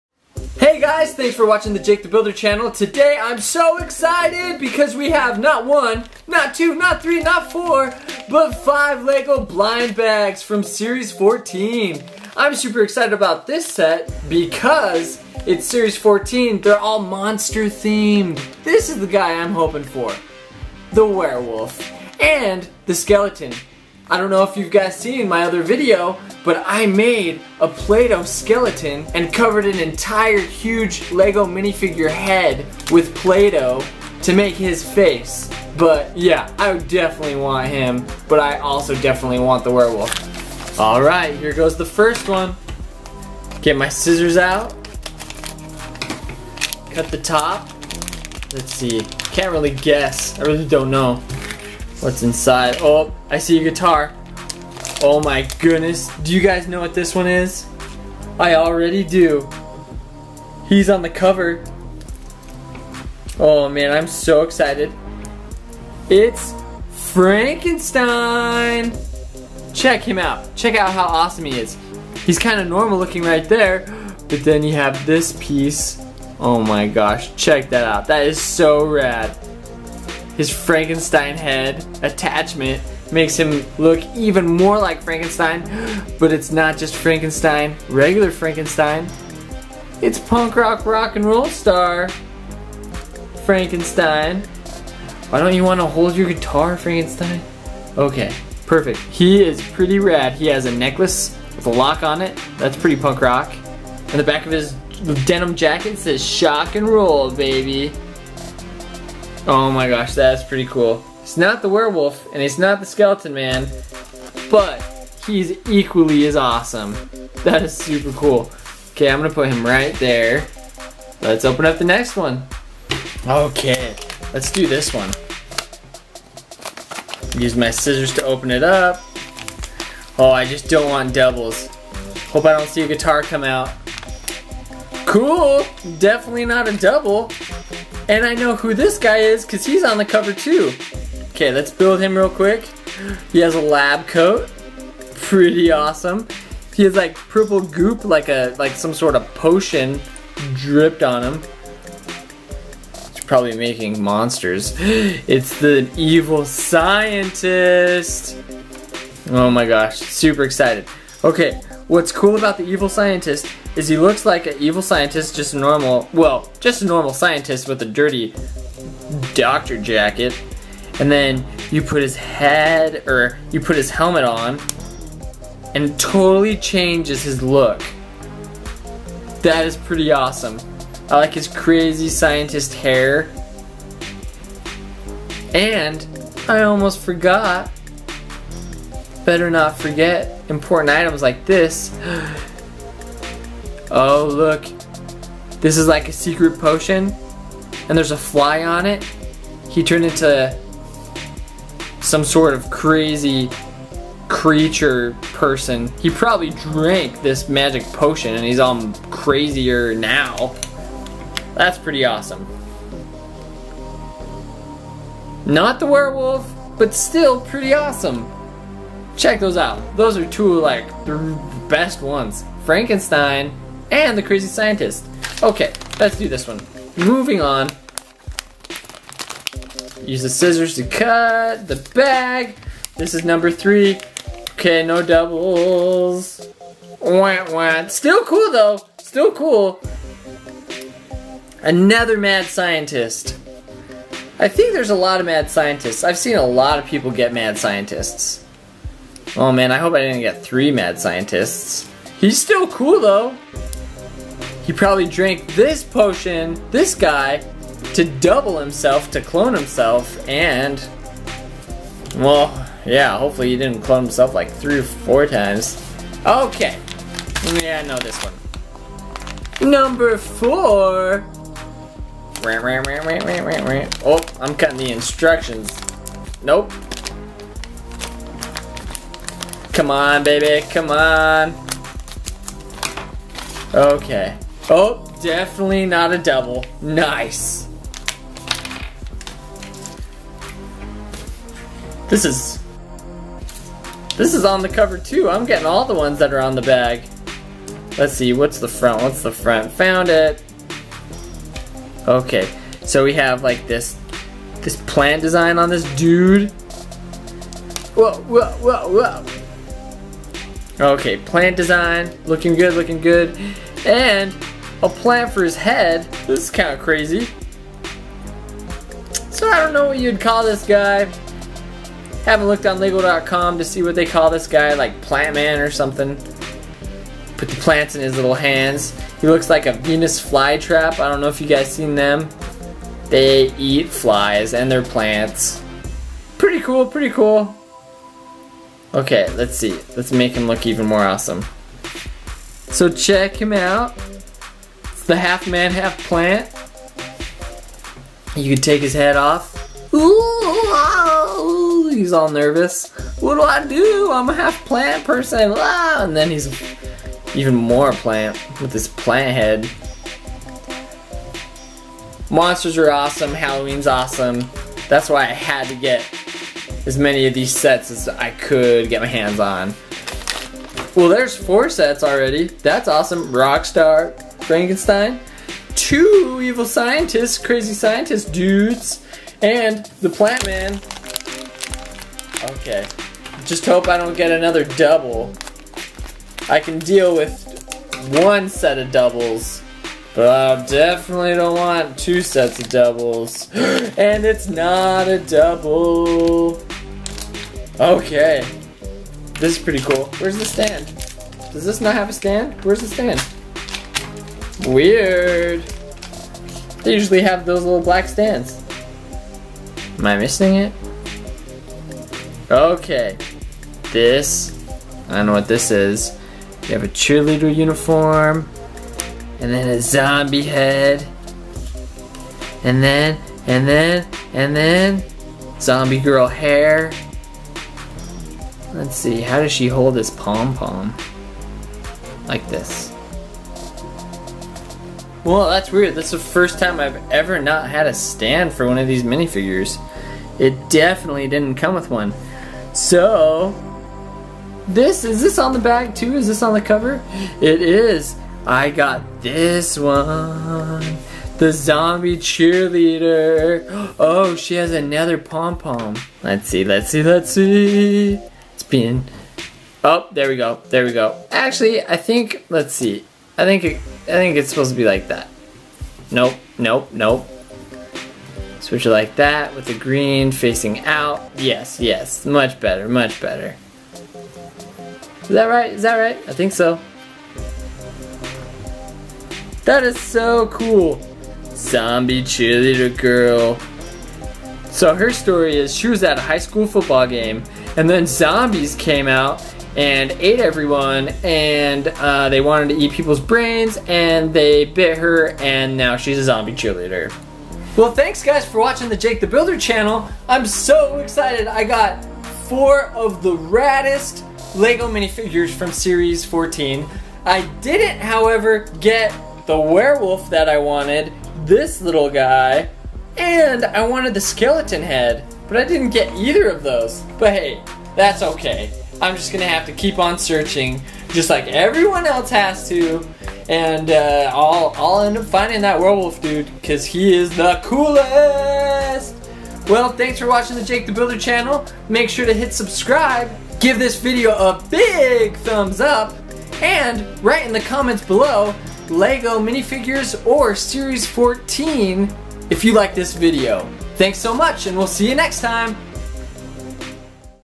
LEGO SERIEs 14 BLIND BAGS with Series 14 "Monster" Minifigures 71010
Today we open up 5 Lego Series 14 Minifigure Blind Bags! Series 14 is so awesome since the whole Series is Monster themed! We get some pretty awesome Minifigures!

Also, let us now in the comments below what else you'd like us to build!

Do you like Speed Builds? Do you like Star Wars?

What's your favorite LEGO® set??? What should I build next???